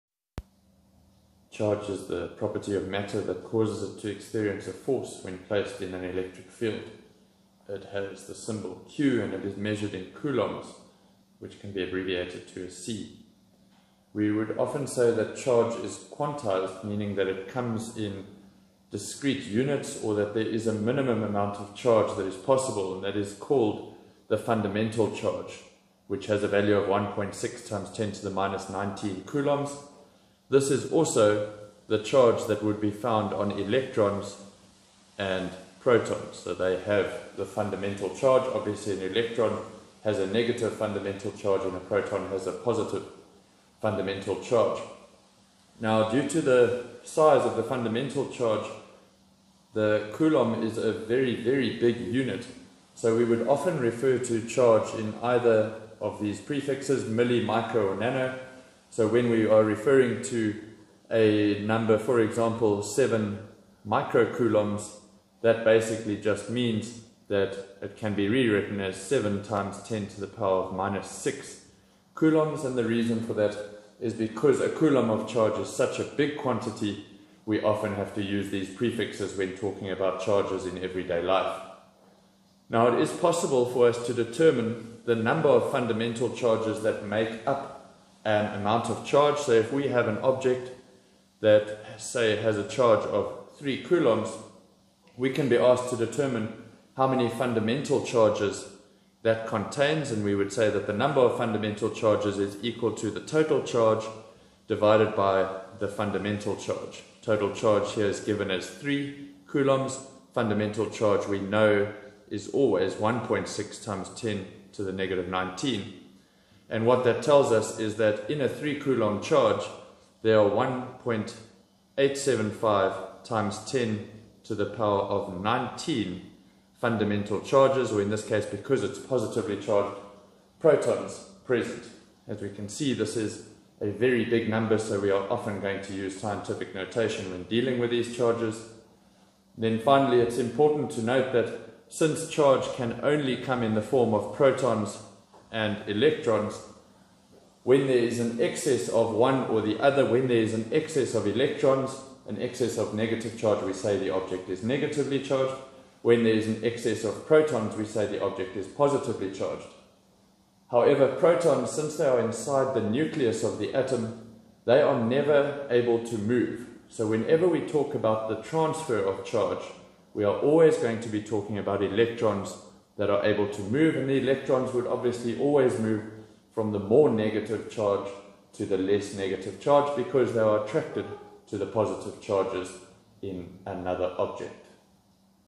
Charge
This video introduces the concept of charge and explains the quantization of charge.

South Africa
CAPS
Grade 8 - 12
Electrostatics